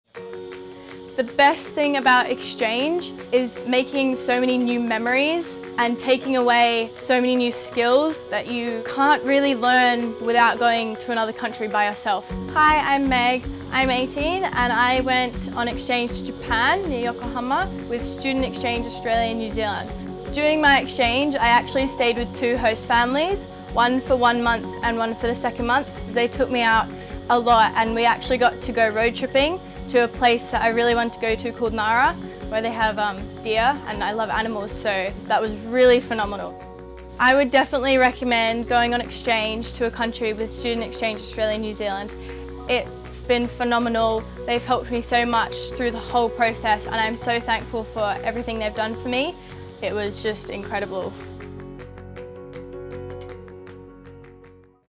Student Exchange to Japan - Meghan
Meghan and her parents talk about her high school exchange program to Japan with Student Exchange Australia New Zealand. for more information on high school exchange programs from Australia and New Zealand.